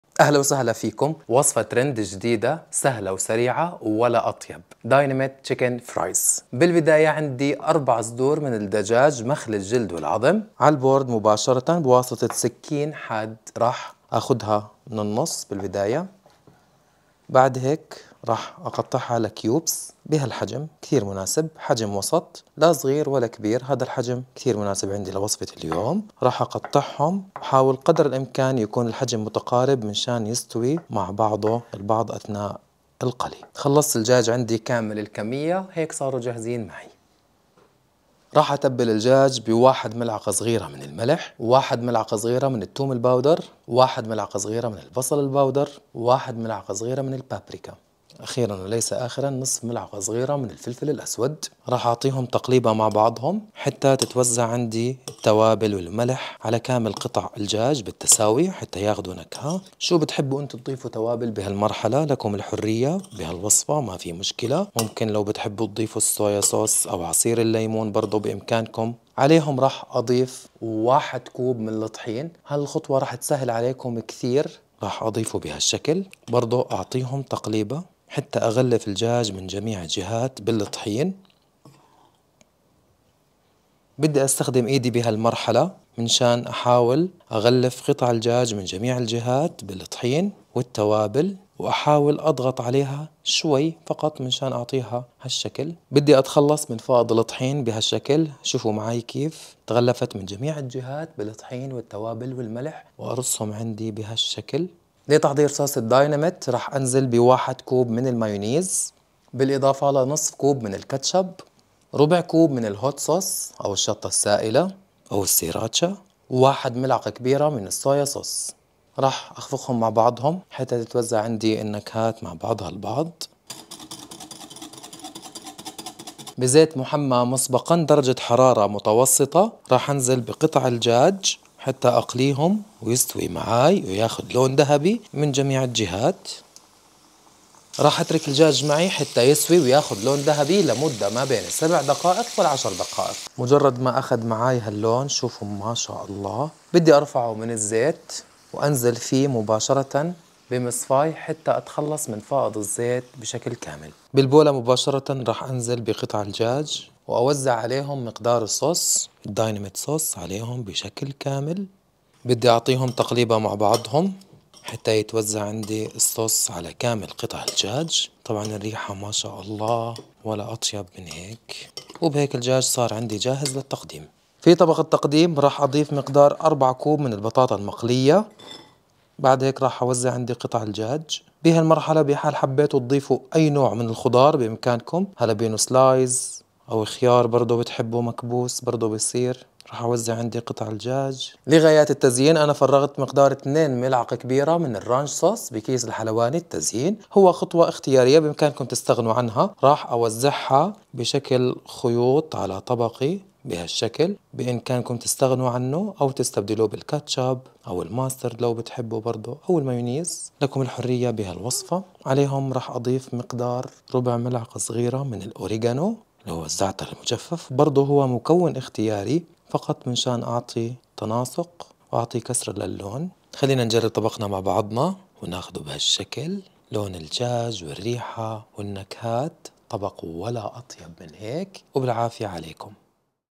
داينميت تشيكين فرايز لذيذ و سهل مع الصوص الشهي 😋😍
مدة التحضير : 30 د
الكميه تكفيي : 4 اشخاص

4صدر دجاج مخلي الجلد والعظم
1م ص ملح
1م ص توم باودر
1م ص بصل باودر
1م ص بابريكا
نصف م ص فلفل اسود
1كوب دقيق
لتحضير صوص الداينمت :
1 كوب مايونيز
نصف كوب كاتشب
ربع كوب هوت صوص
1م ك صويا صوص
للتحضير النهائي :
4كوب بطاطا مقليه
للتزيين :
2م ك  رانش صوص جاهز
ربع م ص اوريغانو
طريقة التحضير:
1-بواسطة سكين حاد نقطع صدور الدجاج بشكل كيوبس
2-نتبل قطع الدجاج بالملح والتوم والبصل الباودر البابريكا والفلفل الاسود ونقلب جيدآ ثم نضيف الدقيق ونقلب ونتخلص من فائض الدقيق ونضع قطع الدجاج على حدى
3-بواسطة مقلاه بداخلها زيت غزير محمى مسبقآ درجة حراره متوسطه نقلي قطع الدجاج حتى تأخذ لون ذهبي لمده مابين
7 - 10د نرفعها من الزيت داخل مصفاه ونضعها جانبآ
4-نحضر صوص الداينمت بواسطه وعاء نضع المايونيز الكاتشب والهوت صوص الصويا صوص  ونخلط جيدآ
5-نضع قطع الدجاج داخل الصوص ونقلب جيدآ
6-في طبق التقديم نضع مقدار البطاطا المقليه نوزع قطع الدجاج بالصوص ثم نزين بالرانش صوص والاوريغانو ويقدم.